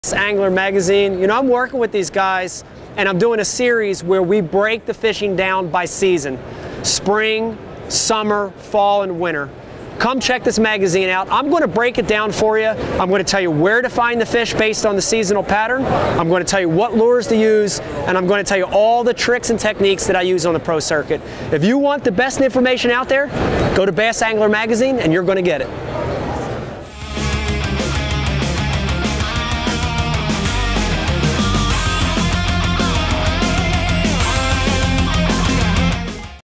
Super Star Bass Pro Mike Iaconelli "Ike" Goes BAM!
Mike Iaconelli talks about the benefits of Bass Angler Magazine and how it will help you catch more fish with every issue.